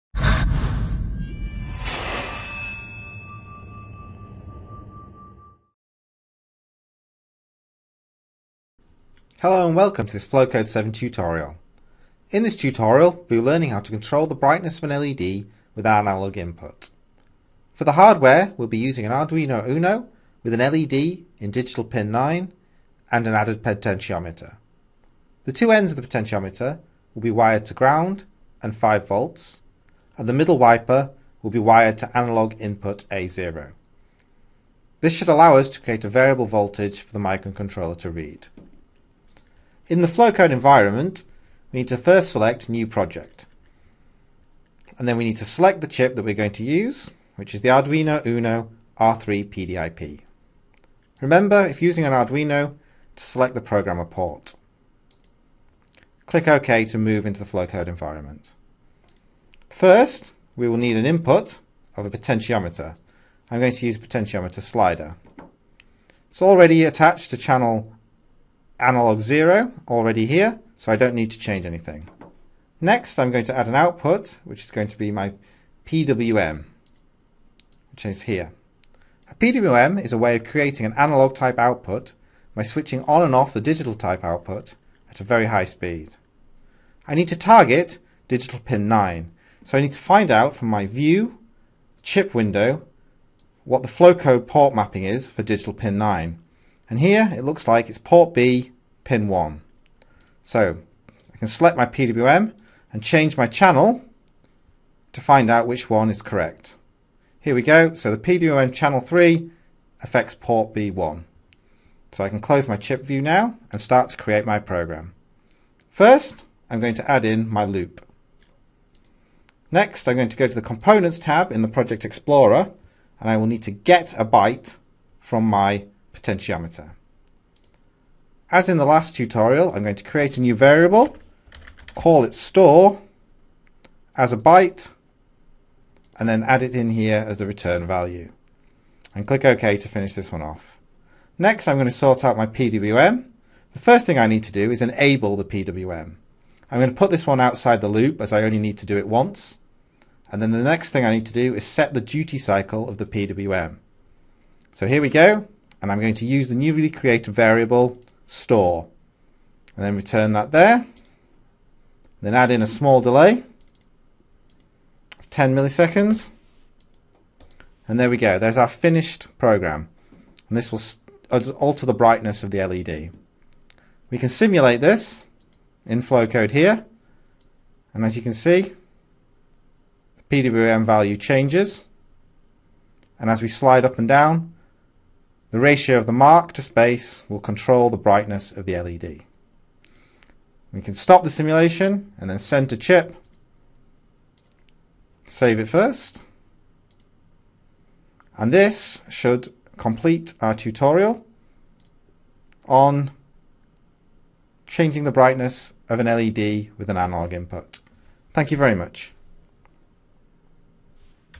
Flowcode 7 Tutorial 4 - LED brightness with an analogue input
A short tutorial on using Matrix Technology Solutions Flowcode 7 program. This is the fourth in a short series of tutorials.

This tutorial is on how to control an LED's brightness with an analogue input.